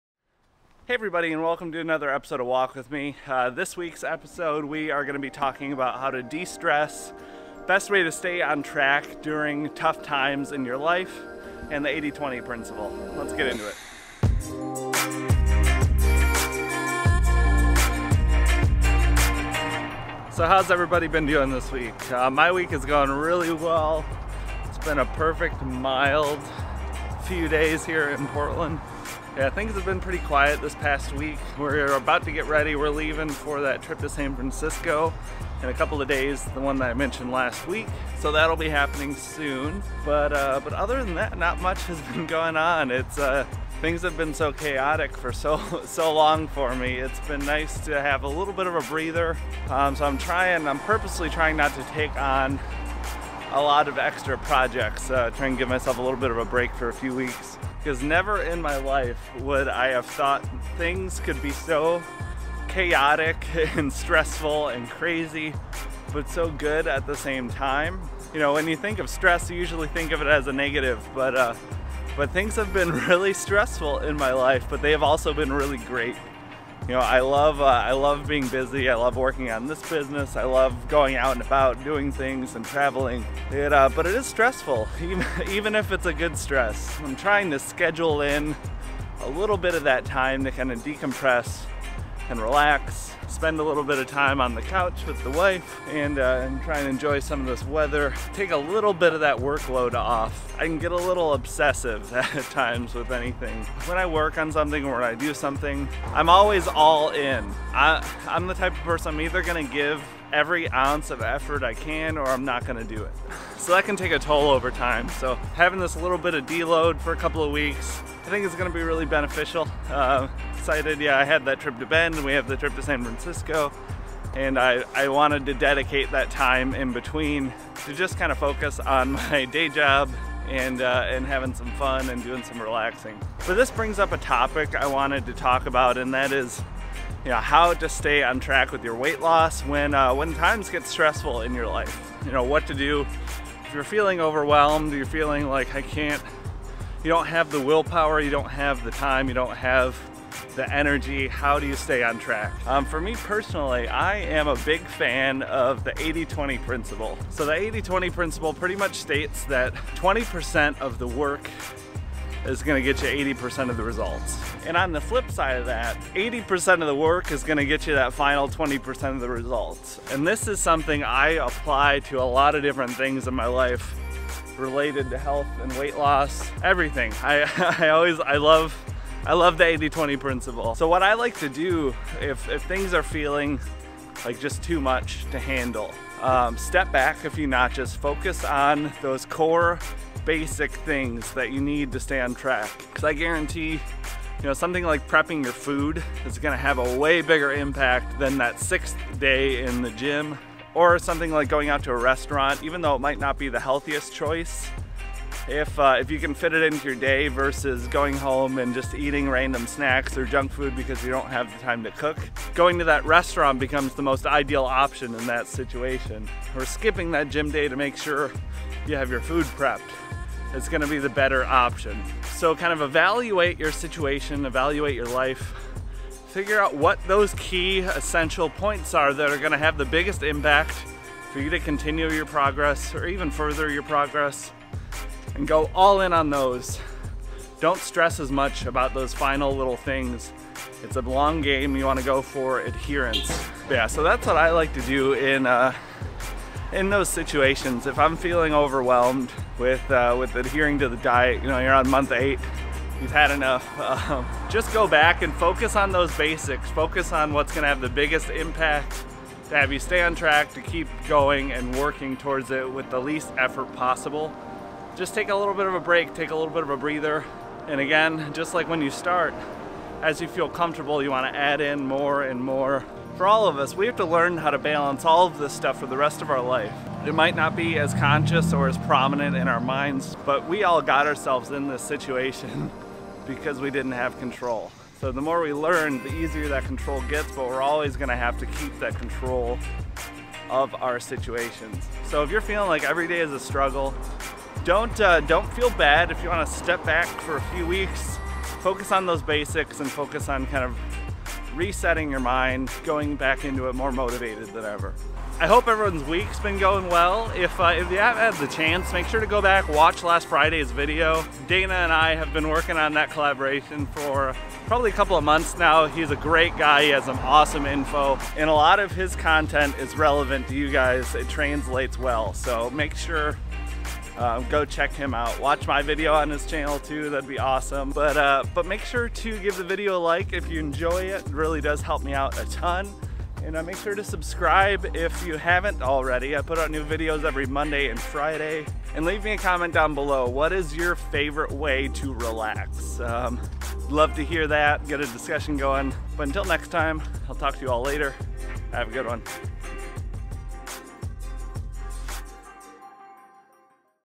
How to Continue Dieting Through Stressful Times
During this week's episode of 'Walk with Me' we discuss how I stay on track with my goals during stressful life events using the 80/20 principle.

GEAR I USE:
Digital Recorder